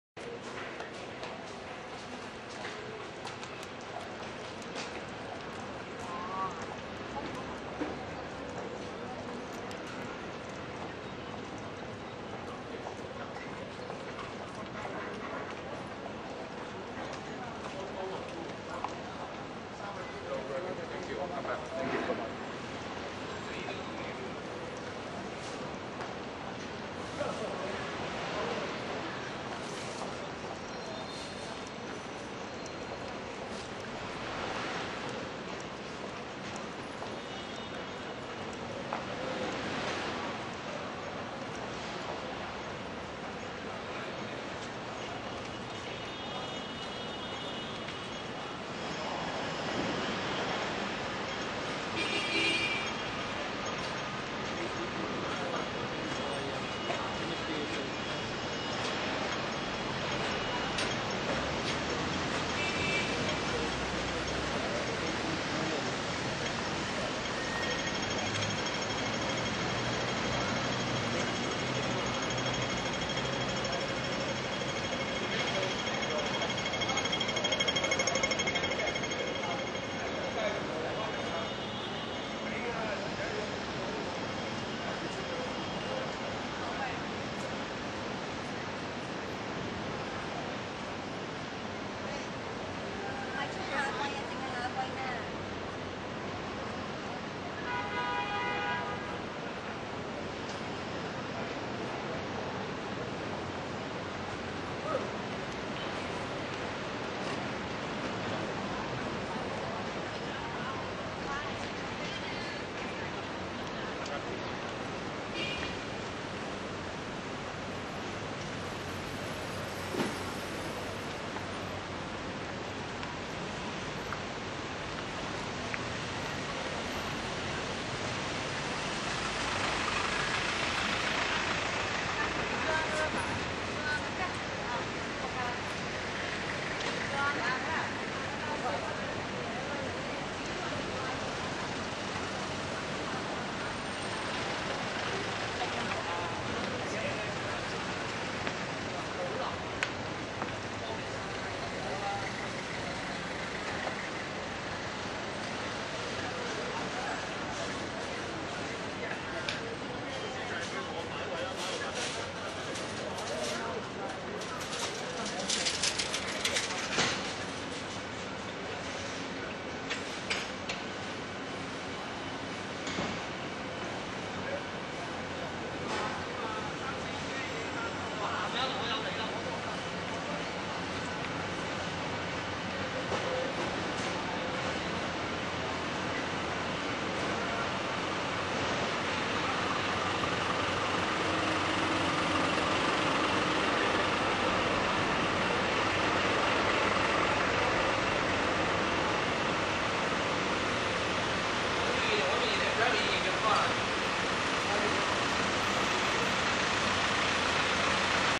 How to get to Whiskey Priest in Lan Kwai Fong from Central MTR
How to get to  Whiskey Priest in Lan Kwai Fong from Central MTR using walkme2